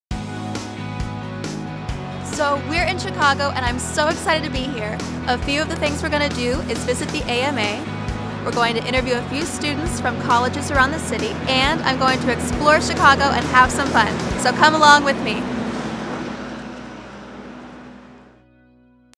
Student Body TV Correspondent Arrives in Chicago
Excited to be in Chicago, our correspondent is looking forward to visiting the AMA (American Medical Association), interviewing college students, and exploring the city!